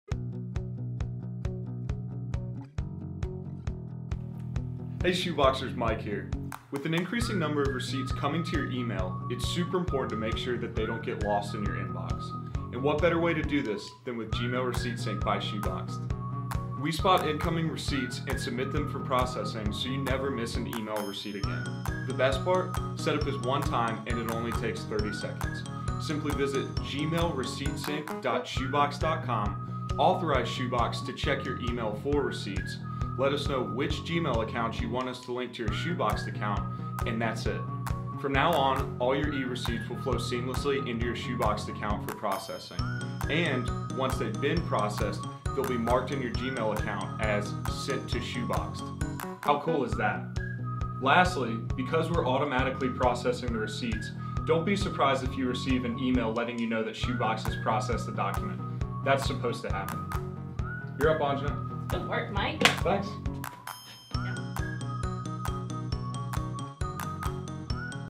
How to Set Up Gmail Receipt Sync
Watch this short video on how to link your Shoeboxed account to your Gmail so all of your e-receipts will flow seamlessly into Shoeboxed for processing.